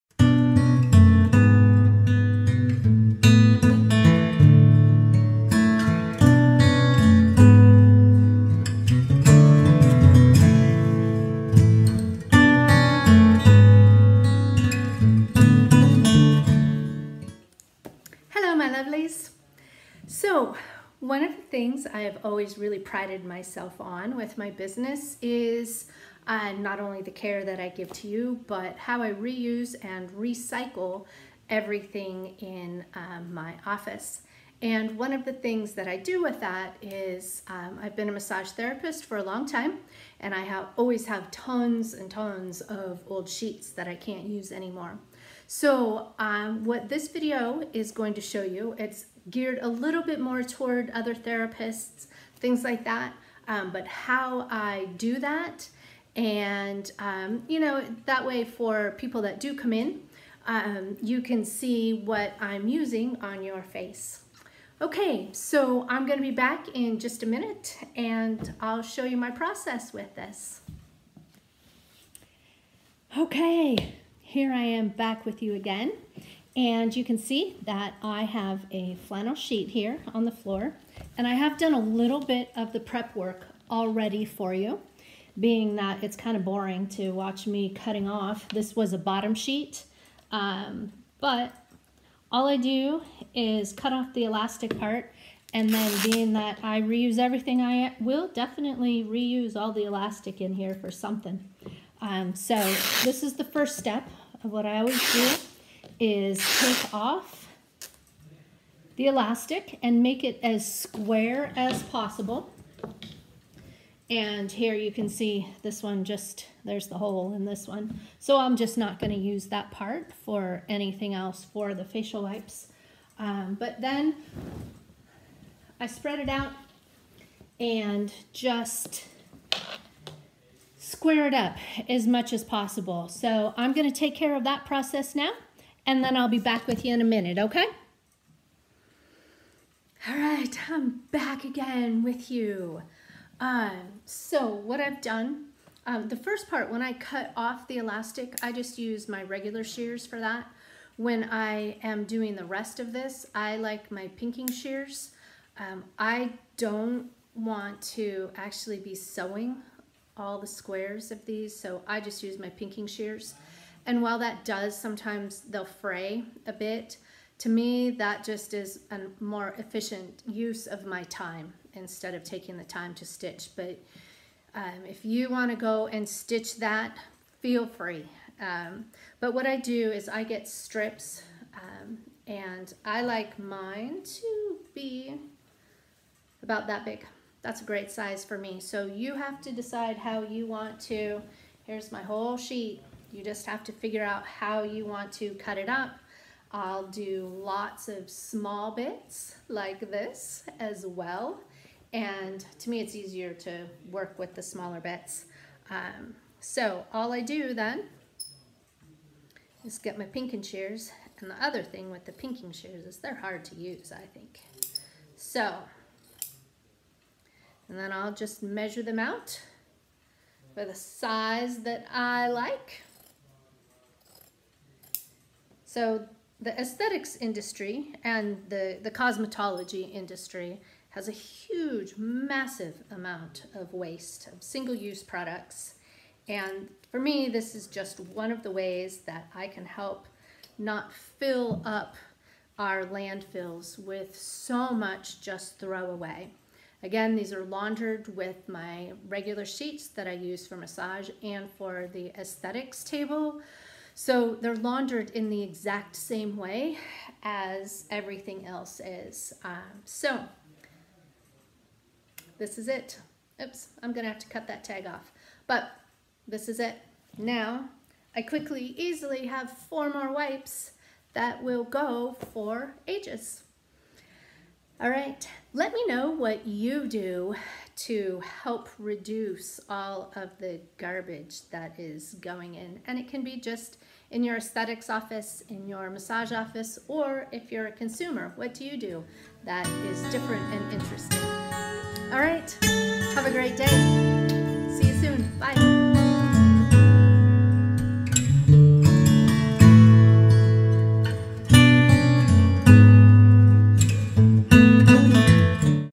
How to make your own facial cloths out of flannel
Best facial cloths for anyone that washes their face.
Perfect for cosmetologists and estheticians. An easy way to reuse and repurpose flannel sheets into facial cloths.
I have found these to be far more gentle on your skin as well. Make some for your personal use too.